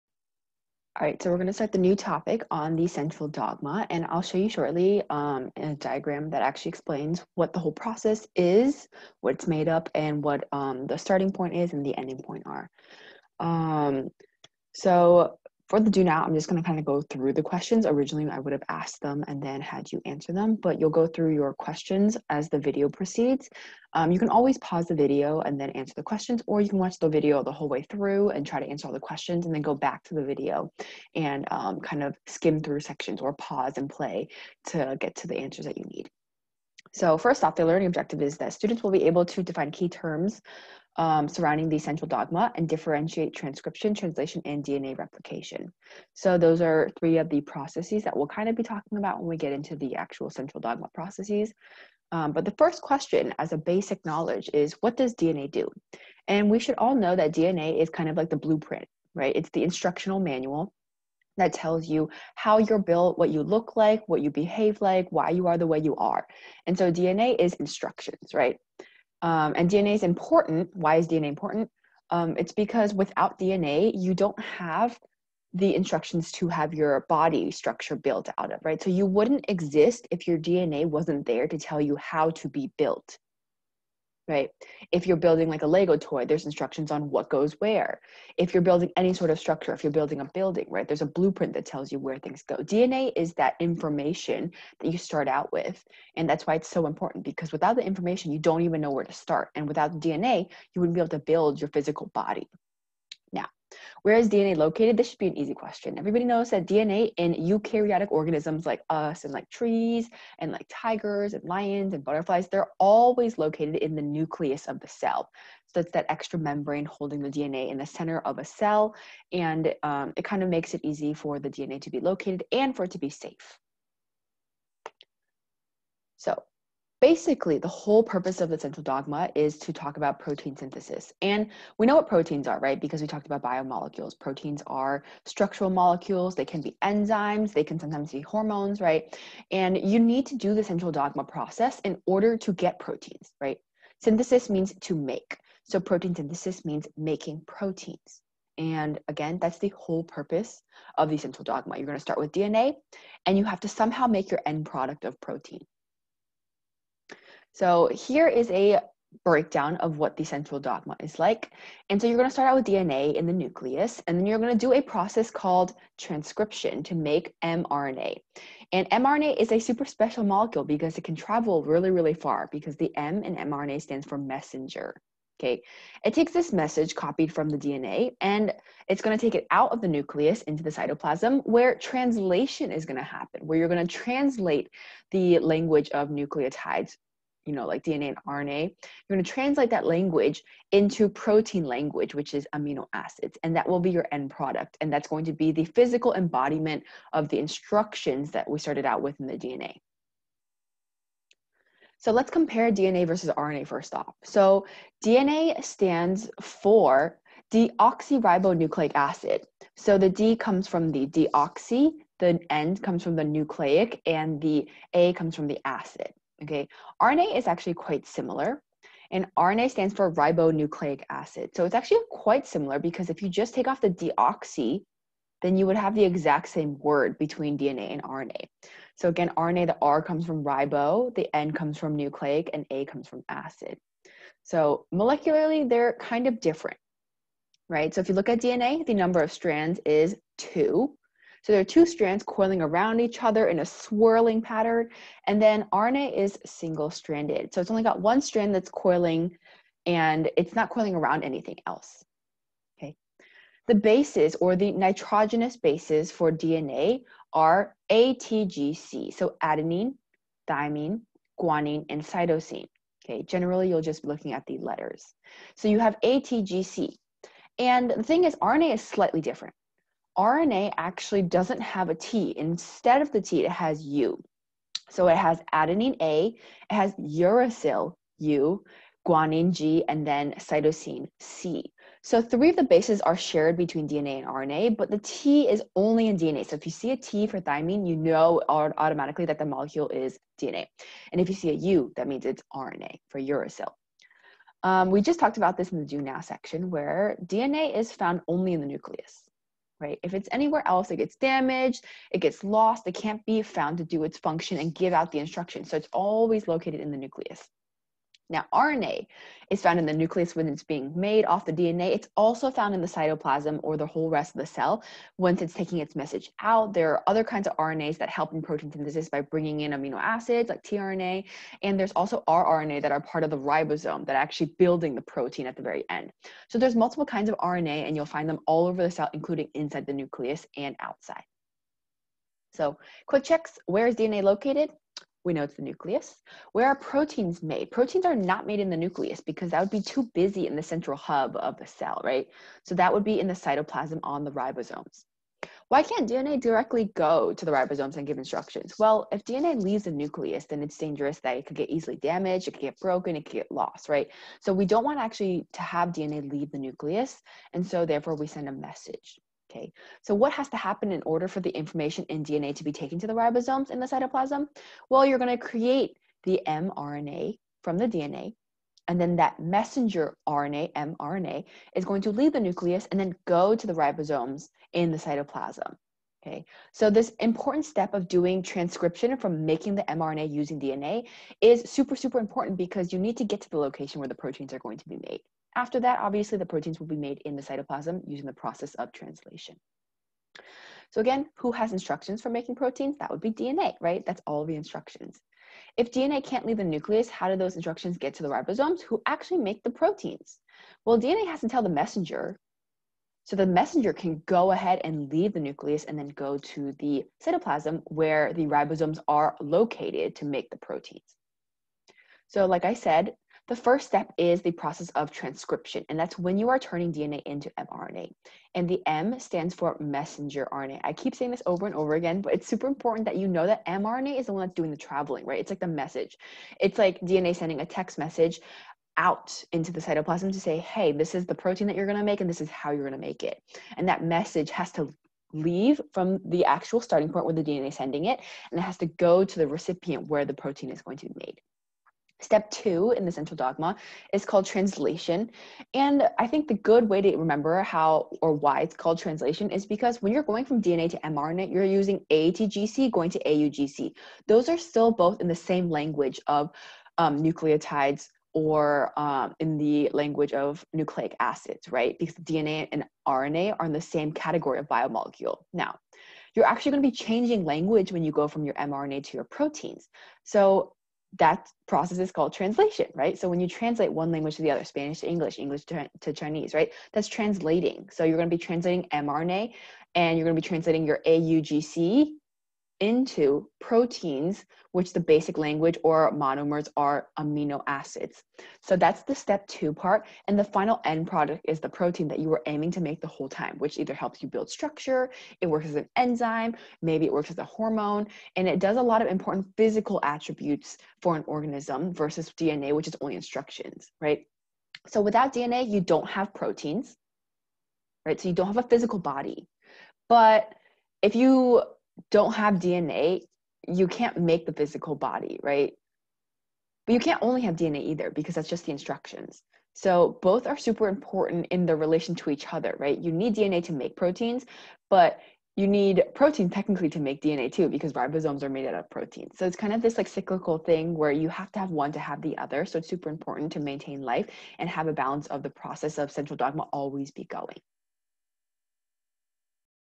Central Dogma Introduction
Here we review the main purpose of completing the central dogma.